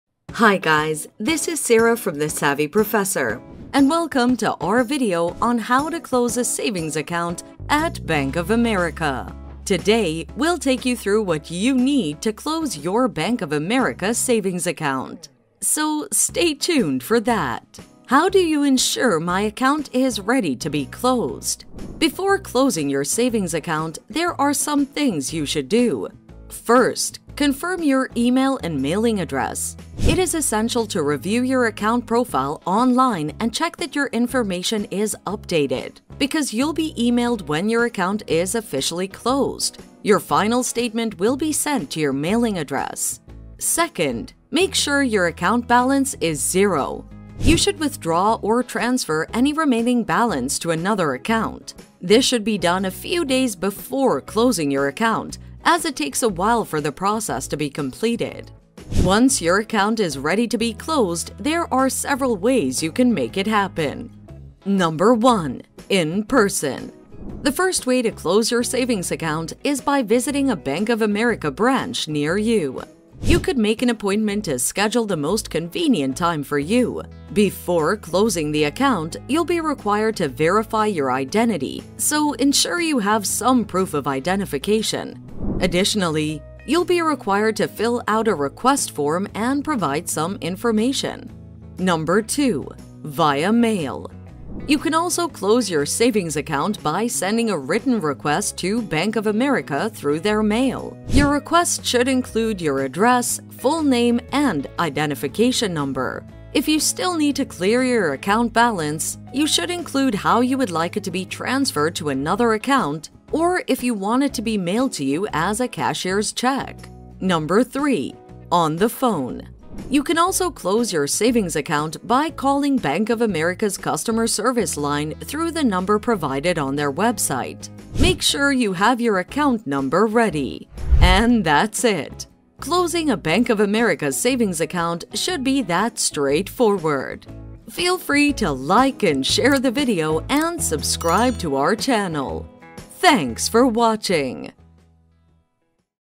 How to Close Savings Account Bank of America (How to Close a Bank of America Account)
How to Close Savings Account Bank of America (How to Close a Bank of America Account). Welcome to our video on how to close a savings account at Bank of America.

How do you ensure my account is ready to be closed?
Before closing your savings account, there are some things you should do:
i.) Confirm your email and mailing address – it is essential to review your account profile online and check that your information is updated because you will be emailed when your account is officially closed. Your final statement will be sent to your mailing address.

ii.) Make sure your account balance is zero – You should withdraw or transfer any remaining balance to another account. This should be done a few days before closing your account, as it takes a while for the process to be completed.